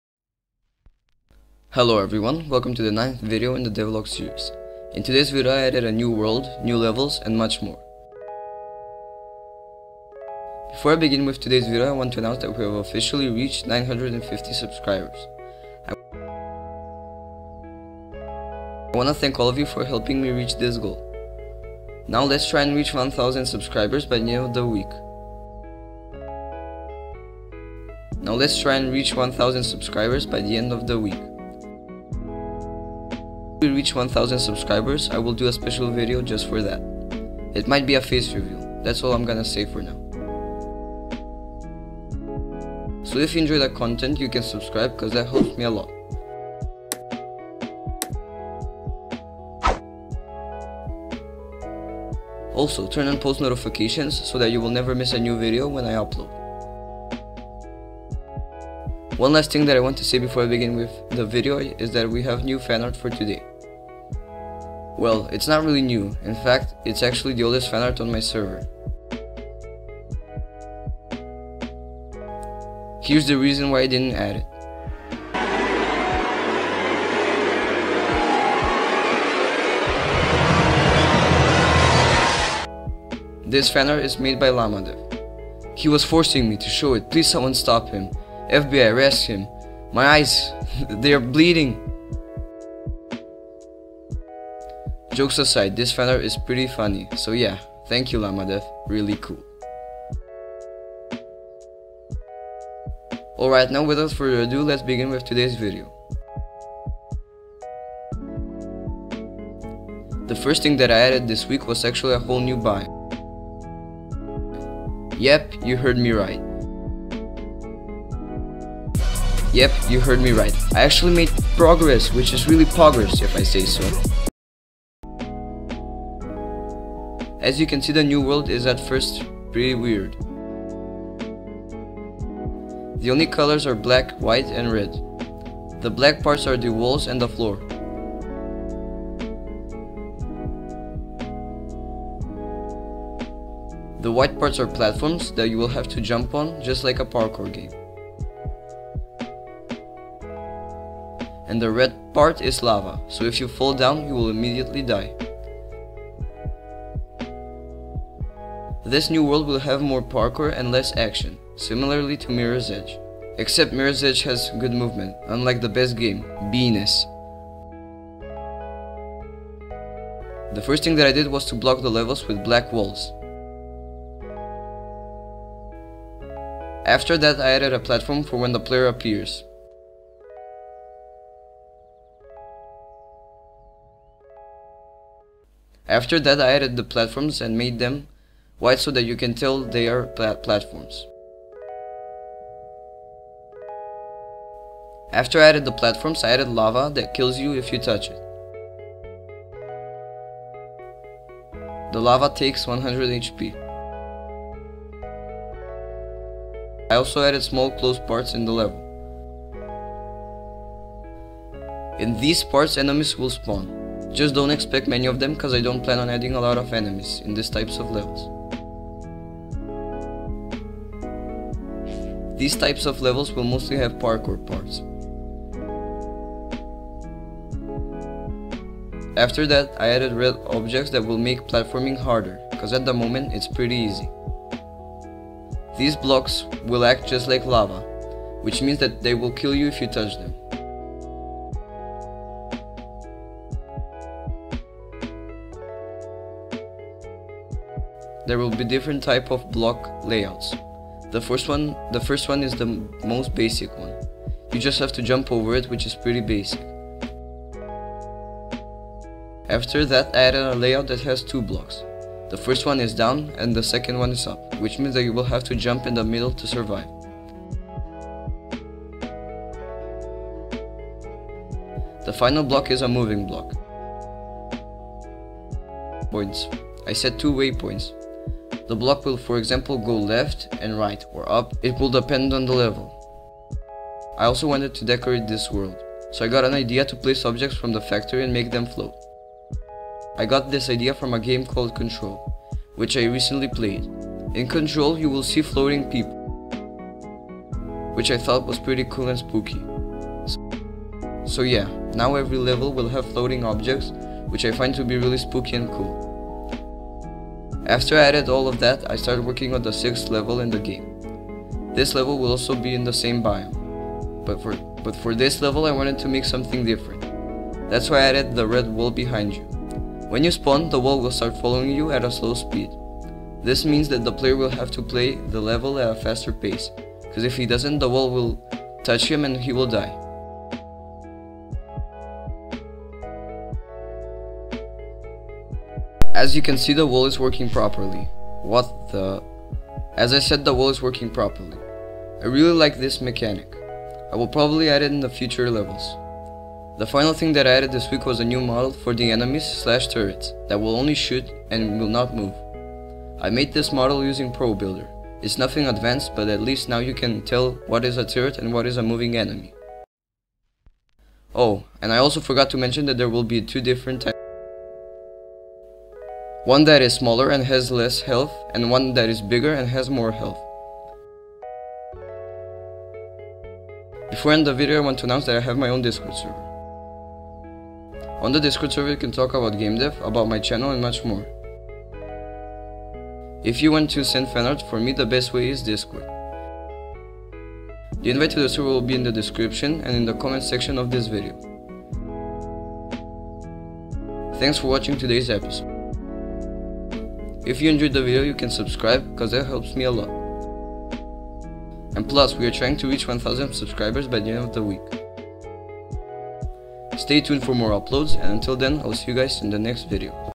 Indie 3D Game Devlog #9 - New World, New Levels & more...
Hello everyone. In todays video i will be making my first 3d game in Unity.